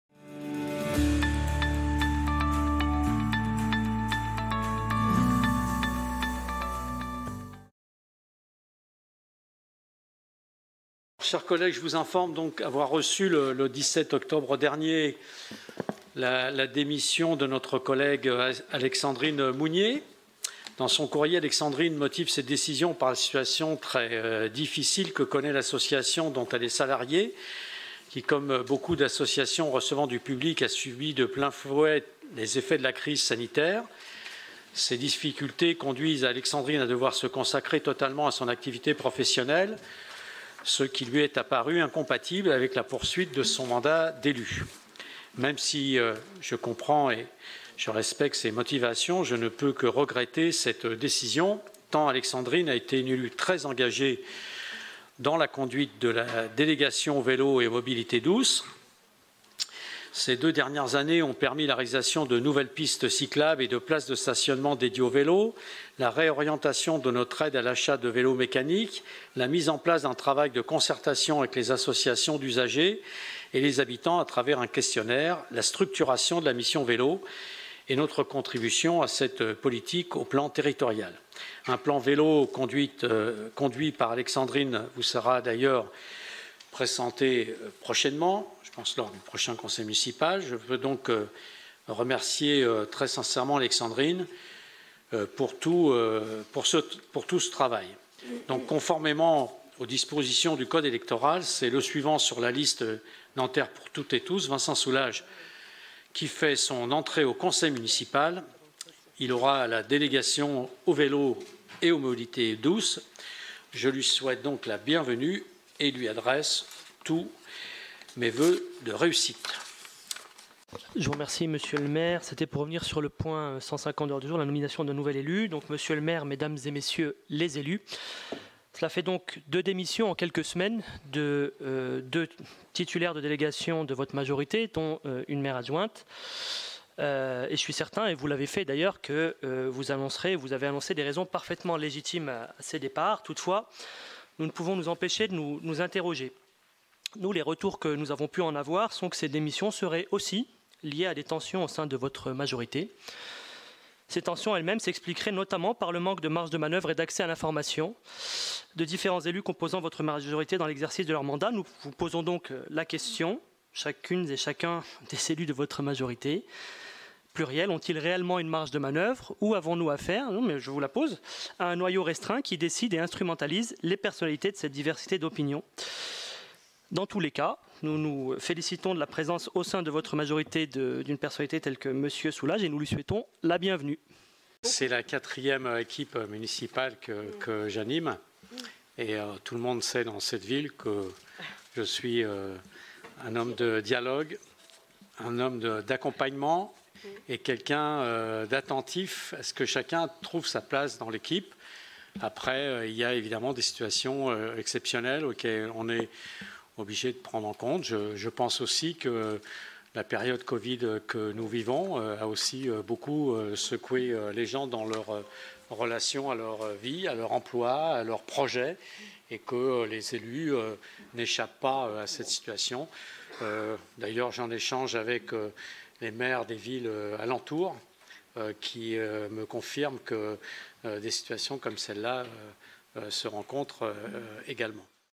Partie 1 - Conseil municipal du 6 décembre 2021
Installation de Vincent Soulage, nouveau conseiller municipal

Suite à un problème technique en début de séance, seule la bande son du conseil est disponible.
Nous vous prions de nous en excuser.

À venir dans les prochaines jours :

Élection de Lise Cortes, 3ème adjointe au maire
Vente d’un bien communal à l’Institut Ibn Badis
Stratégie territoriale de sécurité et de prévention de la délinquance
Budget participatif : convention avec des associations porteuses de projet
Construction d’un lieu culturel et de locaux associatifs au pied des tours Aillaud
Dénomination de voies dans le secteur des papèteries de la Seine
Questions de l’opposition